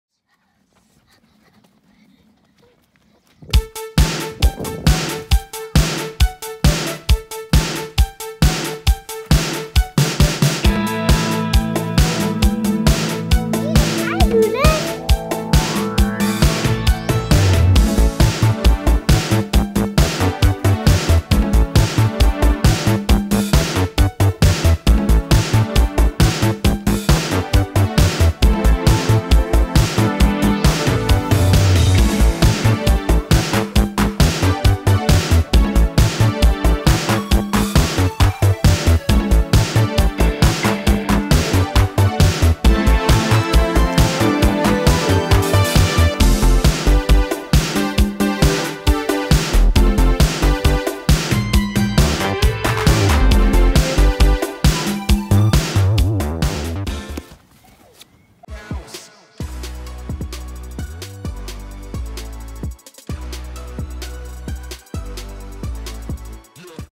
80s Synth-Pop Judith
Sometimes when Judith gets really excited, 80s synth-pop just starts playing.

YouTube disabled our comments : (